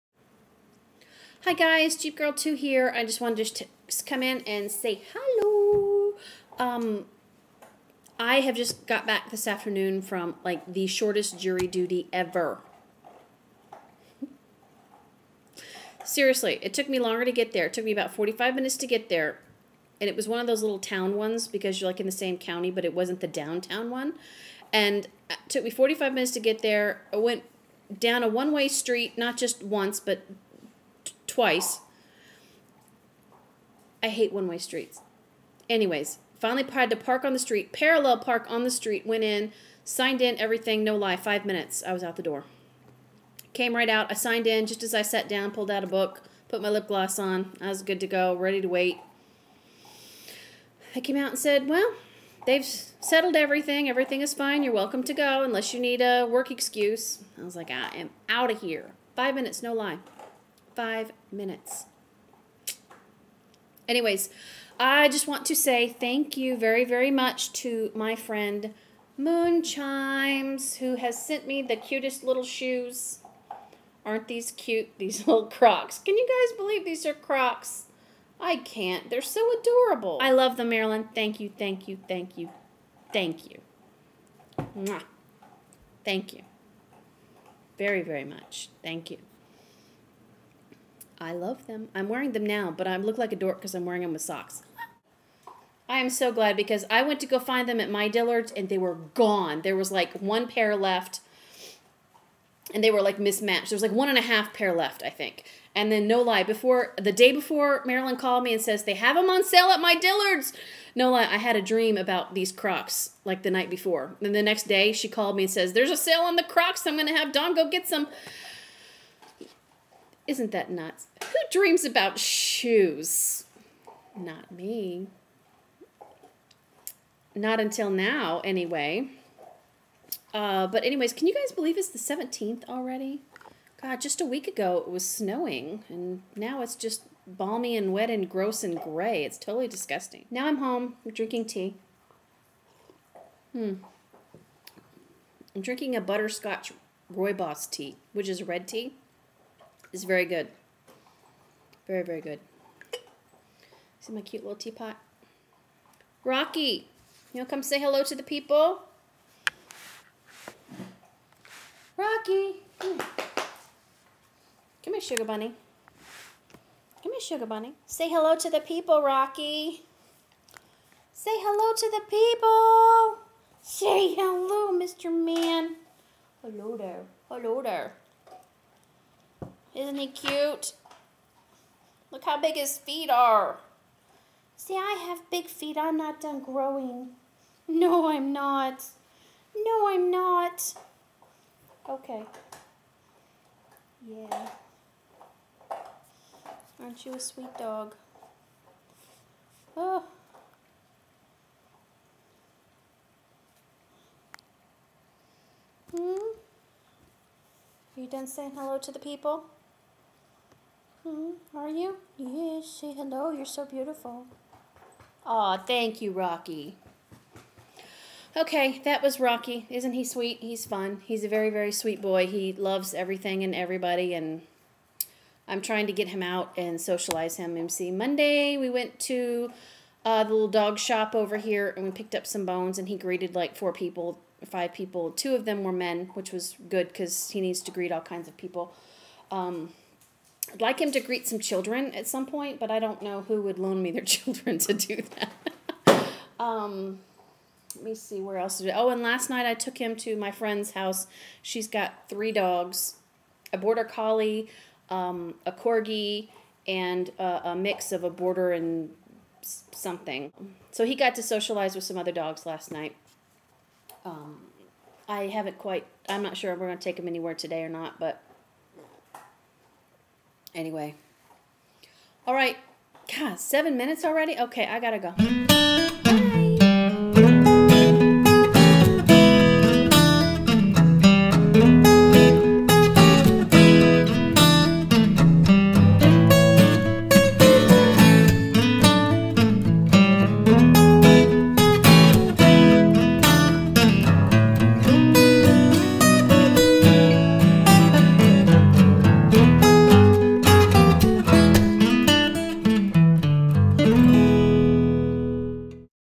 Shoes!!! And a new foster dog.
I got the shoes Marilyn sent me.
Had a very short Jury Duty today; Amazing!
Showing off Rocky my new foster dog
Drinking tea
Snow pictures from last week.